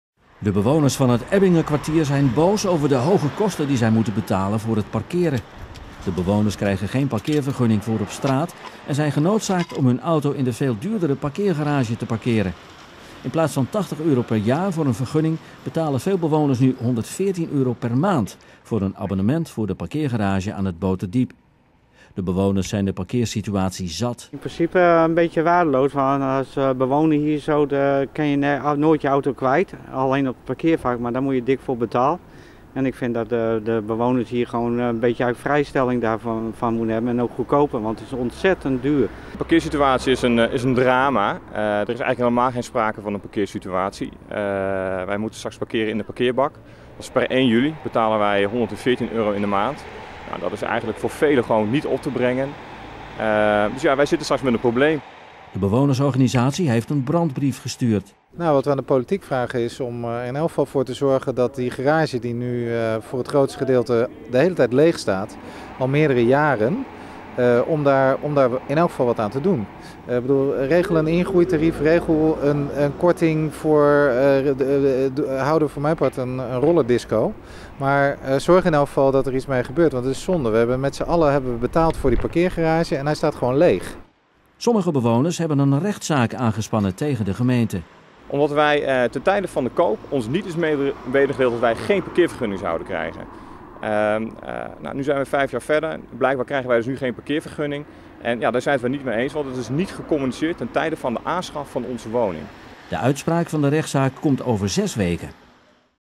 Bewoners Ebbingekwartier boos over parkeerkosten
De bewoners van het Ebbingekwartier zijn boos over de hoge parkeerkosten die zij moeten betalen. Ze krijgen geen parkeervergunning en moeten daardoor gebruik maken van betaald parkeren.Veel bewoners parkeren hun auto in de parkeergarage aan het Boterdiep. De bewonersvereniging heeft een brandbrief aan de gemeente gestuurd waarin zij oproepen de situatie te veranderen. Sommige bewoners hebben een rechtszaak aangespannen tegen de gemeente. Dit omdat hen ten tijde van de aankoop van hun woning niet is verteld dat ze geen parkeervergunning zouden krijgen. de uitspraak van deze rechtszaak komt over zes weken.